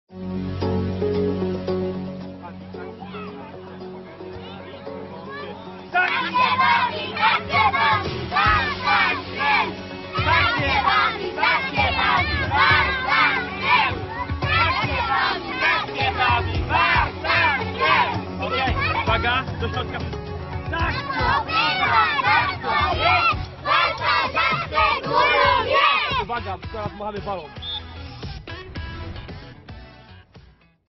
Warta Śrem rocznik 2011
Rocznik 2011 Warty Śrem rozegrał kolejny sparing mający na celu przede wszystkim dobra zabawę. Zabawę , która uczy i rozwija umiejętności piłkarskie. Tym razem zmierzyliśmy się z rówieśnikami z Czempinia.

Pięknych bramek i emocji nie brakowało :)